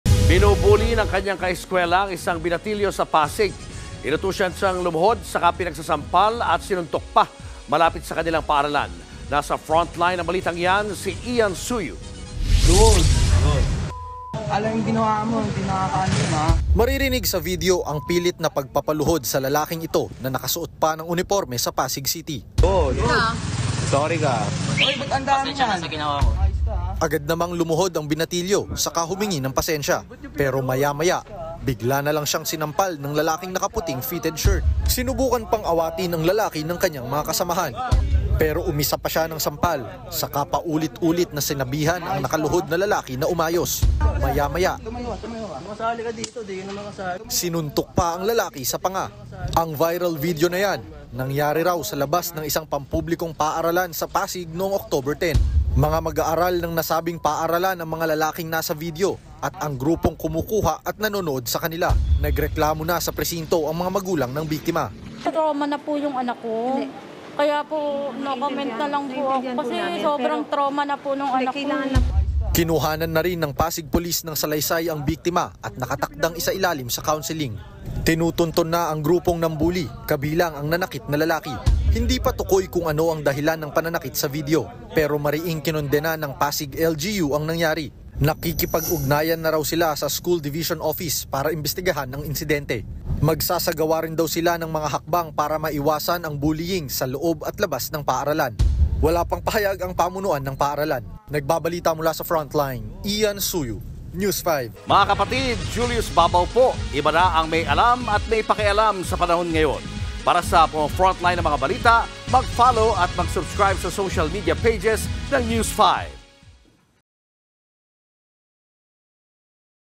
Lalaking estudyante, binully ng mga schoolmate; pinaluhod, pinagsasampal, at sinuntok pa
BABALA: SENSITIBONG BALITA

FrontlinePilipinas | Binu-bully ng kaniyang kaeskuwela ang isang binatilyo sa Pasig City.

Inutusan siyang lumuhod saka pinagsasampal at sinuntok pa malapit sa kanilang paaralan. News5 | via Ian Suyu